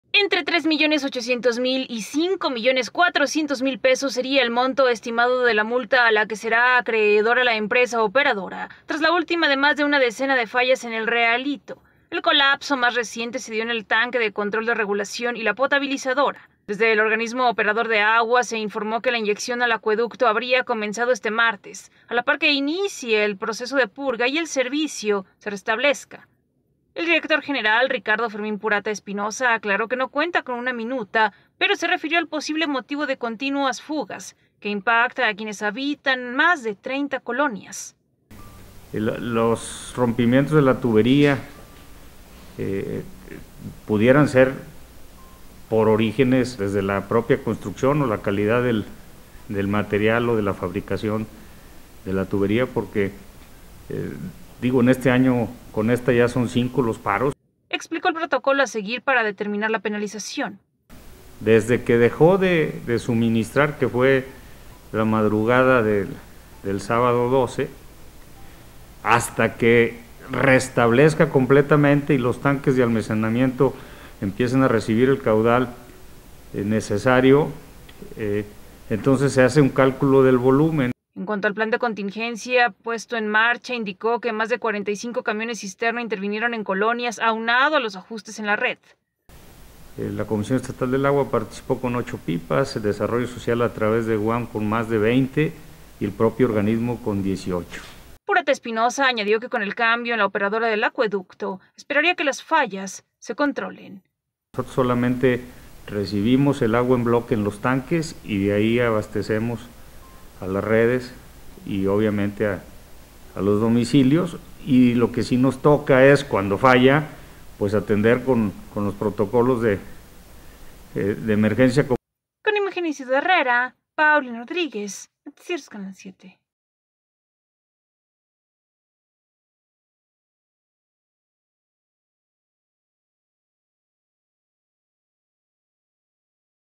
Hasta a cinco millones 400 mil pesos podría ascender la multa por última falla en El Realito.
SLP.- Entre tres millones 800 mil y cinco millones 400 mil pesos sería el monto estimado de la multa a la que será acreedora la empresa operadora, tras la última de más de una decena de fallas en El Realito.

El colapso más reciente se dio en el tanque de control de regulación y la potabilizadora.

Desde el Organismo Operador de Agua se informó que la inyección al acueducto habría comenzado este martes, a la par que inicie el proceso de purga y el servicio se restablezca.

El Director General, Ricardo Fermín Purata Espinoza aclaró que no cuenta con una minuta, pero se refirió al posible motivo de continuas fugas -que impacta a quienes habitan más de 30 colonias-.

Explicó el protocolo a seguir para determinar la penalización.

En cuanto al plan de contingencia pues en marcha, indicó que más de 45 camiones cisterna intervinieron en colonias, aunado a los ajustes en la red.

Purata Espinoza añadió que con el cambio en la operadora del acueducto, esperaría que las fallas se controlen.